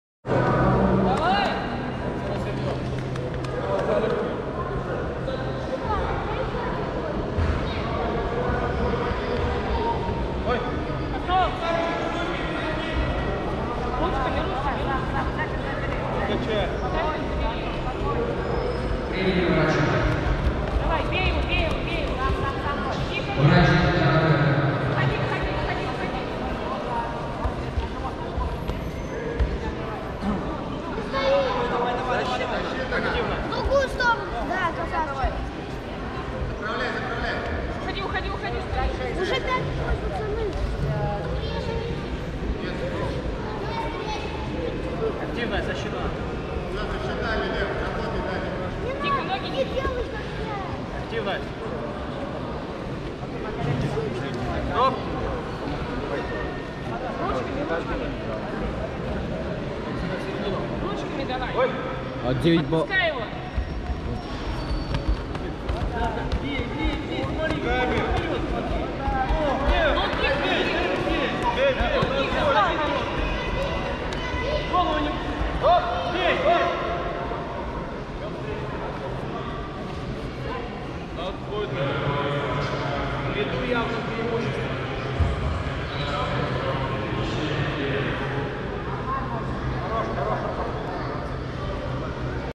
Сличенко Артур (синяя футболка), клуб ЯРОПОЛК, Открытый кубок Московской Обл, ПАНКРАТИОН, 12.06.2021
12 июня 2021 года в г. Троицк Московской области прошел Кубок Московской области по спортивной борьбе- ПАНКРАТИОН, посвящённый празднованию Дню России.

🥊 В турнире приняло участие 100 спортсменов из 17 команд Москвы, Московской, Воронежской, Липецкой, Смоленской, Тульской и Ярославской областей.
🏆 Сборная команда СПК «ЯРОПОЛК» завоевала на прошедшем турнире 3 первых, 3 вторых и 2 третье мест.
В общекомандном зачёте клуб "ЯРОПОЛК" занял 2 место.